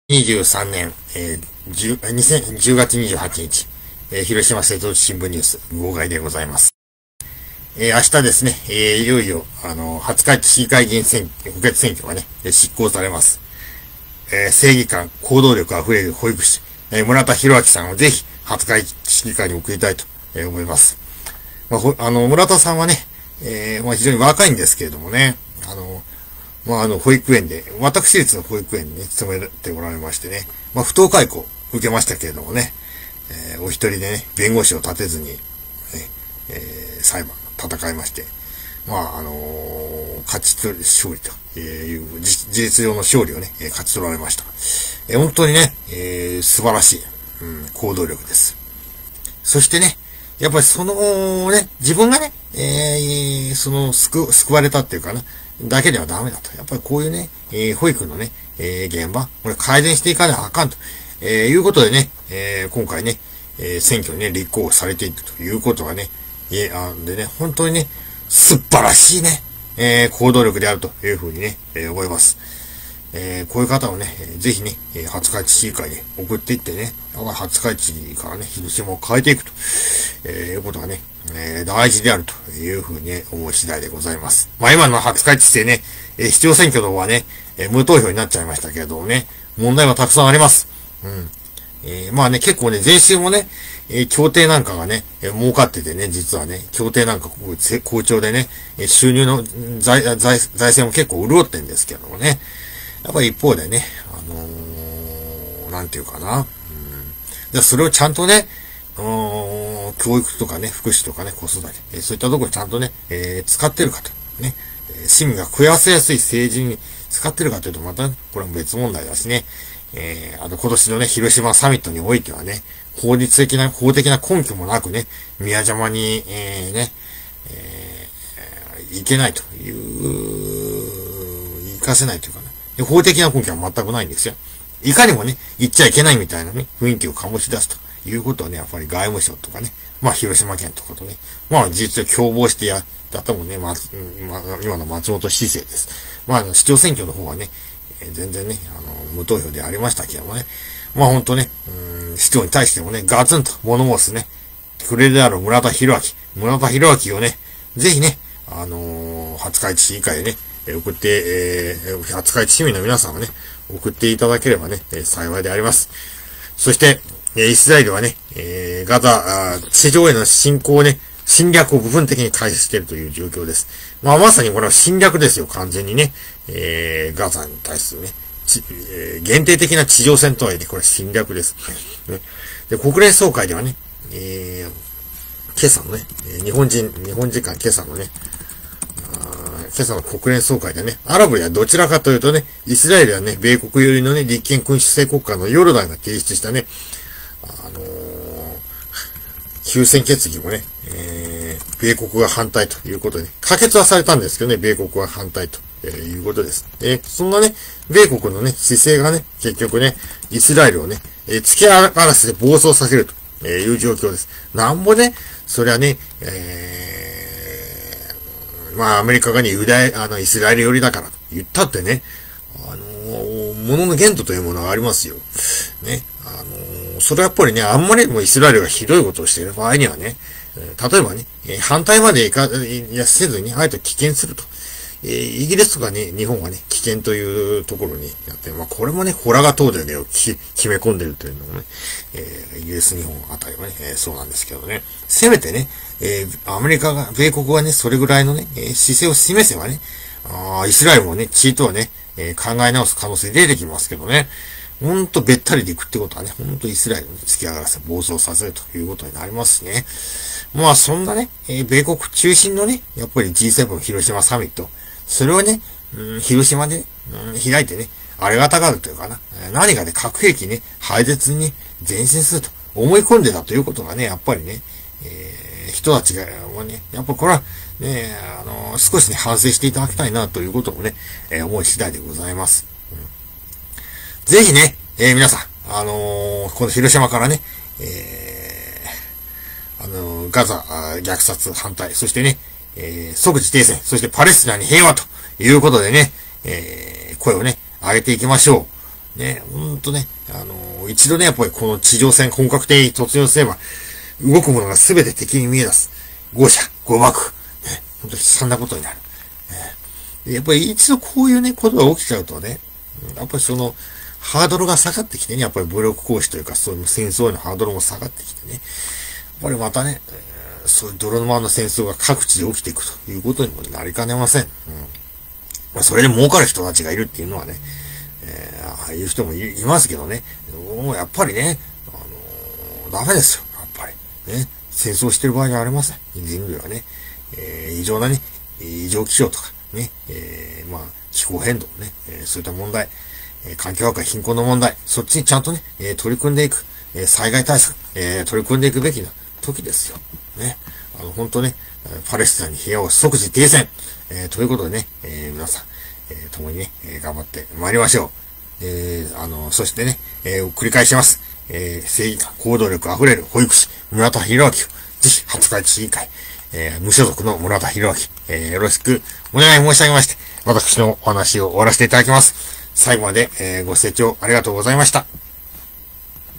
広島瀬戸内新聞ニュース号外10月２８日 村田ひろあきさんを廿日市市議会へ！／ガザ虐殺を止めさせ、即時停戦を！
廿日市市議会議員補欠選挙
正義感・行動力溢れる保育士・村田ひろあき　さんを廿日市市議会へ！
イスラエルがガザ地上への侵略を部分的に開始。
日本時間今朝の国連総会ではアラブではどちらかといえばイスラエルや米国寄りの立憲君主制国家のヨルダン提出の休戦決議可決もまたまた米国が反対。
そんな米国の姿勢がイスラエルの暴走を後押し！
とにかく、ガザ虐殺を止めろ！
即時停戦！パレスチナに平和を！

10月27日（金）の報告と「ガザとは何か」11.2 緊急トーク

◆毎日誰かしら、夕方に原爆ドーム前にいます。
　金曜日のマイクアピールありのスタンディングに参加できない方は、17:30以降の時間
　帯をめどにドーム前を目指して下さい。
　（もし時間差で会えなかった場合に備え、お一人または同行者と一緒にスタンディング
　できるご用意をして参加されてもいいと思います）。

◆今度の木曜日の緊急トークイベント「ガザとは何か」のお知らせです（添付ファイルあり）
　北海道から手弁当で東京・仙台などを周り、広島にも来られるジャーナリストの小田
　切拡さんの話を伺います。
　原爆ドーム前でのスタンディング後に移動しやすい場所として、ライブハウスOtis!を
　会場にしました（徒歩の場合は平和公園を南北に抜け、平和大通りを渡ってさらに南下、
　広島文化学園HBGホールの向かい側になります）。

【日時】11月2日（木）　開始19:00（オープン：18:30）※21:30頃まで

　　　　　広島駅からのアクセス：広島駅より24番のバス「広島市文化交流会館前」
　　　　　（旧厚生年金会館）下車。タクシーだと10分程。
　　　　　予約不要・参加費￥500（学生無料）＋ワンドリンク制　（ドリンクは600円～です）。

【報告者】小田切拓（ジャーナリスト）　（聞き手：田浪亜央江／広島市立大学教員）

　　　　　小田切拓：70回以上のパレスチナ滞在、20年以上の取材経験を持つジャーナリスト。
　　　　　雑誌『世界』 （岩波書店）に「ガザ―人間の壊し方」（2004） 、
　　　　　「ハマスの６ヶ月―民主主義が瓦解する」（2006）、
　　　　　「誰が、得をするのか－「ガザ戦争」長期化の理由」（2014）など、
　　　　　パレスチナと援助、対テロ戦争との関連性等についての論考を２０本以上寄稿。
　　　　　岡真理、早尾貴紀との共編訳書にサラ・ロイ『ホロコーストからガザへ』（青土社、2009）がある。